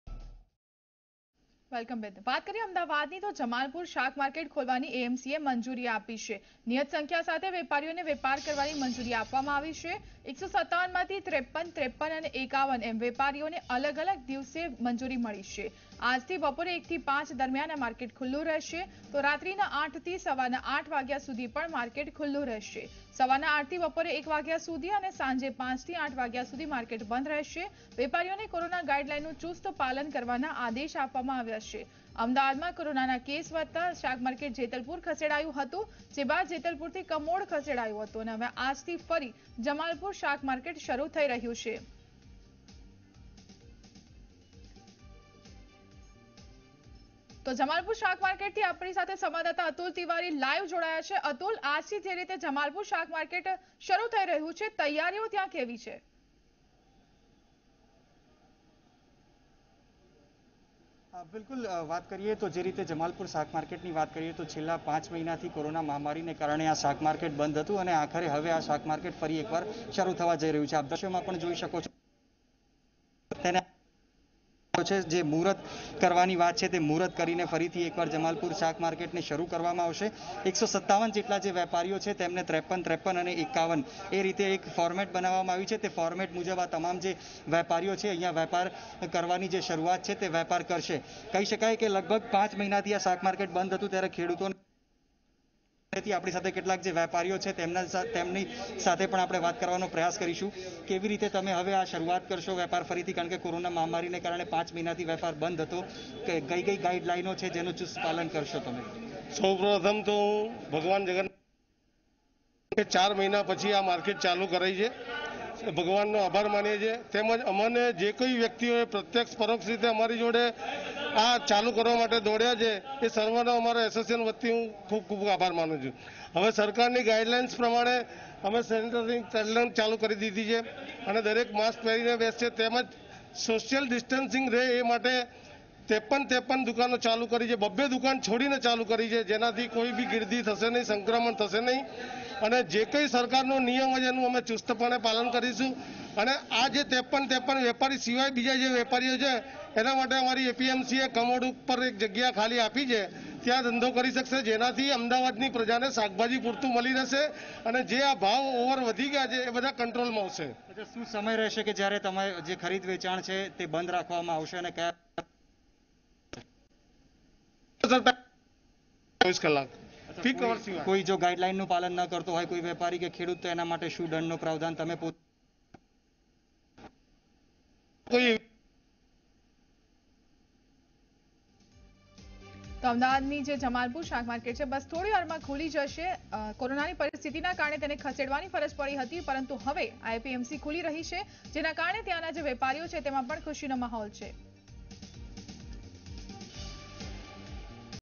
શાકભાજીના ભાવ હવે કંટ્રોલમાં આવશે | Jamalpur Vegetable Market opens | Zee 24 Kalak
સતત વધી રહેલા શાકભાજીના ભાવ હવે કંટ્રોલમાં આવી શકશે. અમદાવાદના જમાલપુર શાકમાર્કેટને ખુલ્લું રાખવાની મંજૂરી મળતા હવે આજથી માર્કેટ ખુલી ગયું છે. વધુ વિગતો માટે જુઓ વીડિયો.